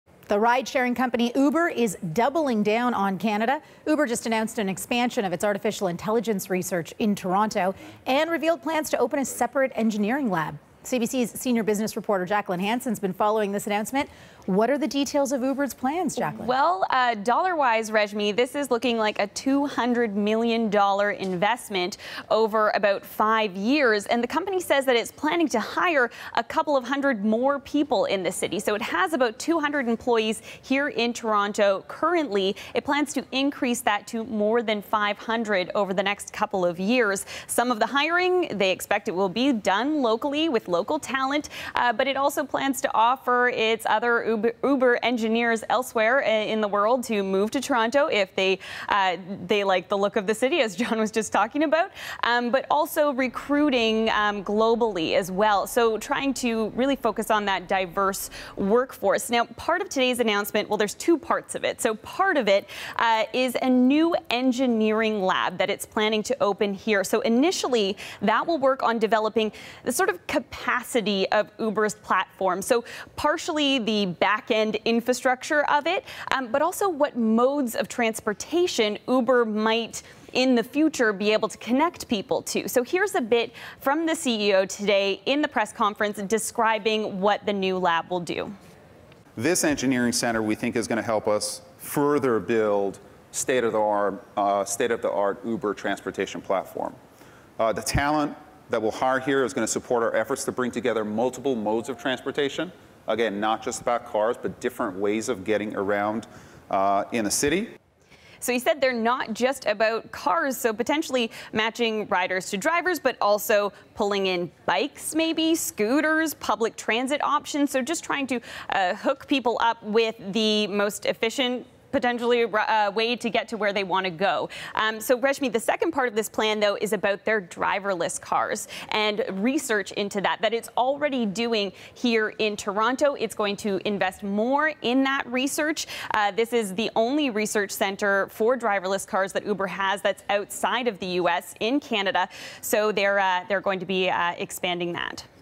Uber expands self-driving car research in Toronto and plans new lab
Uber has announced it will be expanding its self-driving car research group in Toronto and opening a new engineering lab — its first in Canada. Uber says it plans to spend $200 million on the Toronto hub over the next five years.